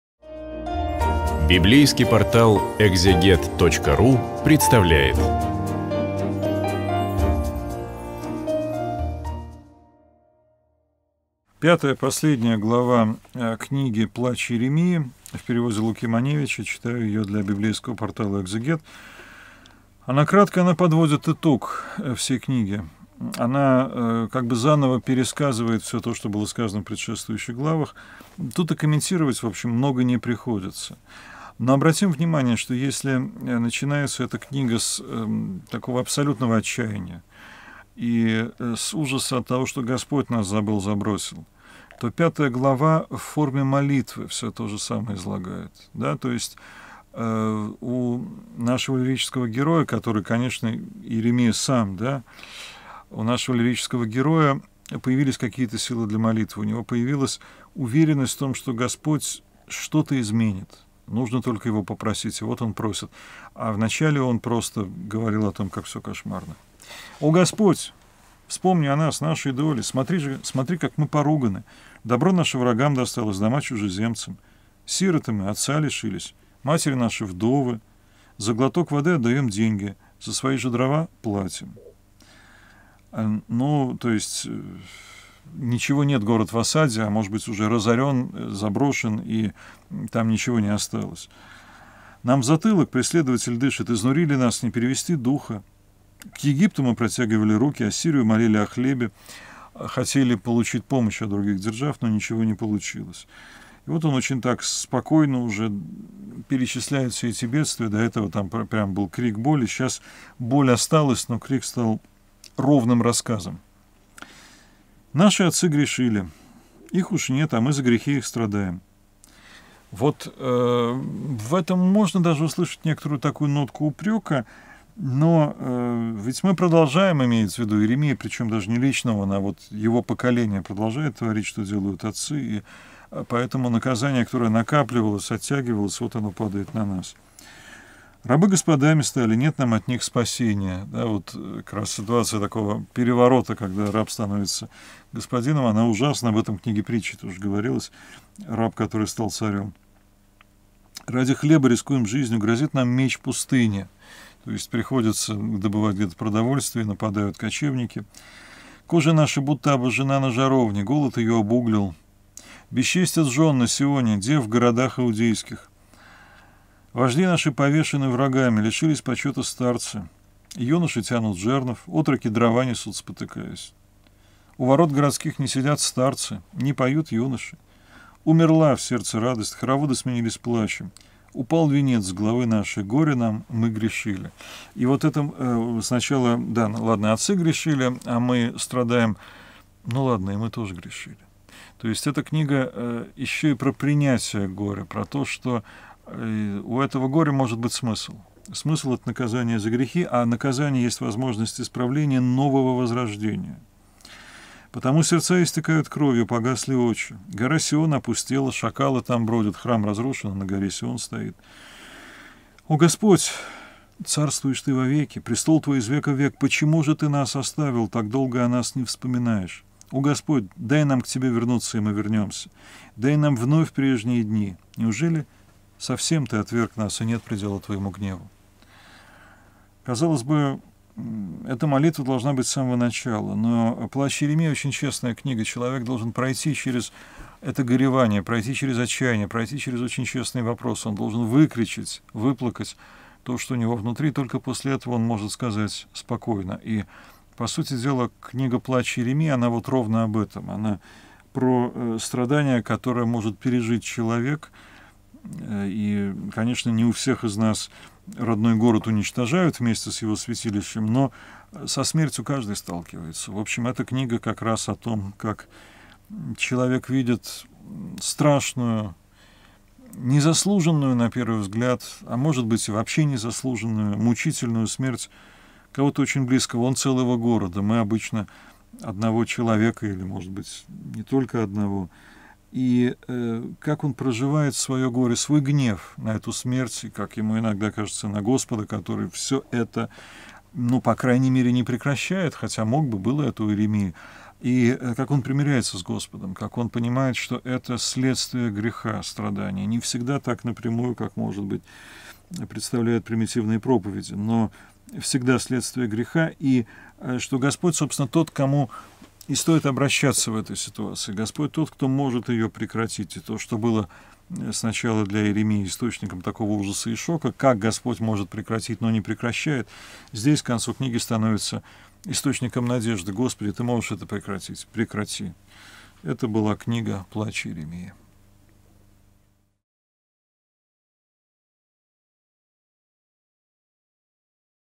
Плач Иеремии. Глава 5. Профессор Андрей Десницкий. Библия
Книга Плач Иеремии с трудом поддаётся комментированию - это глубокое духовное переживание пророка, чьи пророчества исполнились.

5 глава - молитва к Богу, в которой Иеремия изливает свою скорбь. В этой молитве от лица израильского народа появляется и надежда на изменение, на очищение и восстановление.

Разбираем 5 главу книги Плач Иеремии с профессором Андреем Десницким